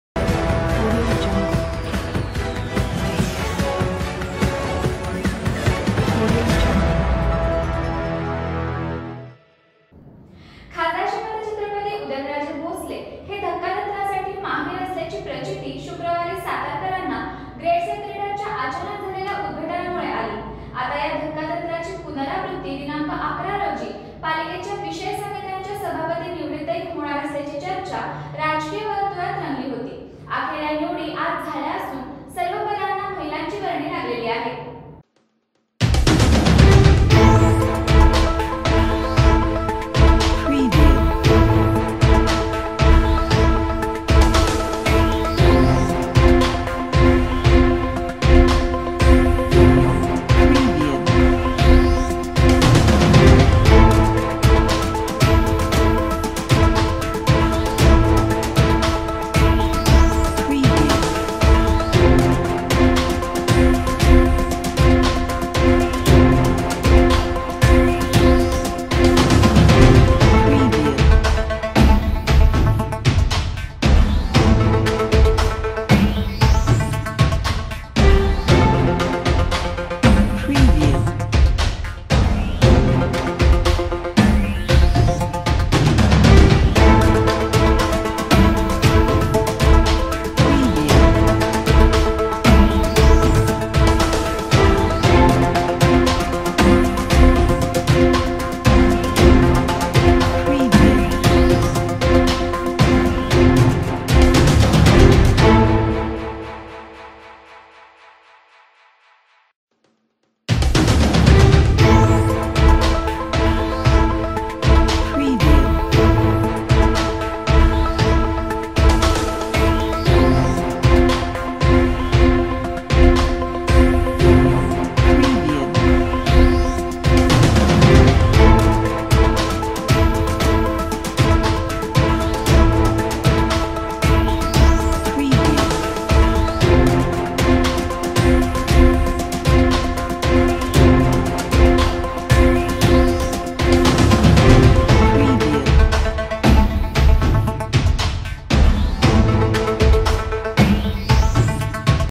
धक्कातंत्राची पुनरावृत्ती पालिकेच्या सभापती निवडीतही : सर्व पदांवर महिलांची वर्णी
सातारा न्यूज